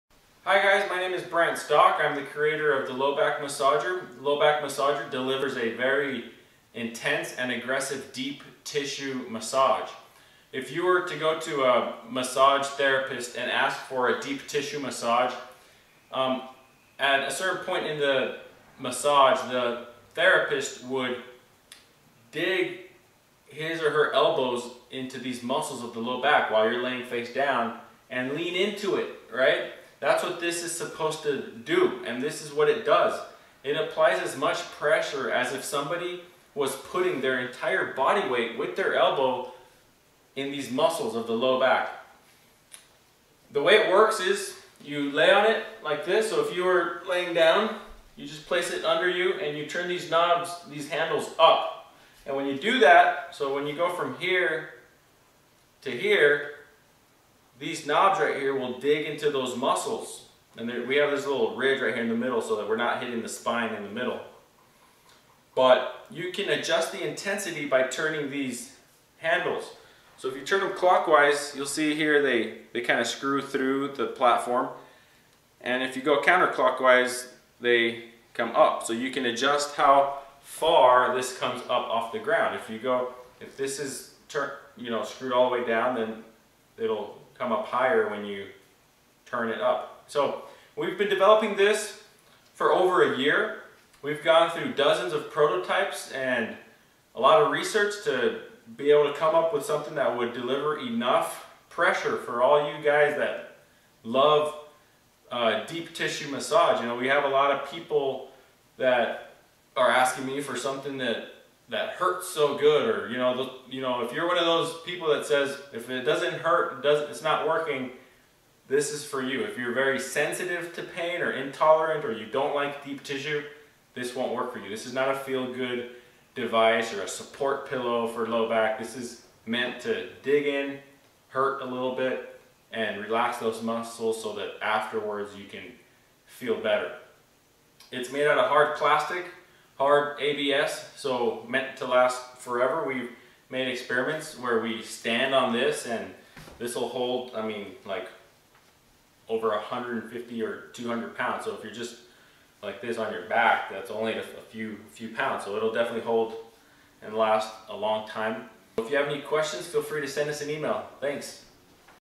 Dr. Brant describes The Low Back Massager™
Get it at thedeeptissuedoc.com.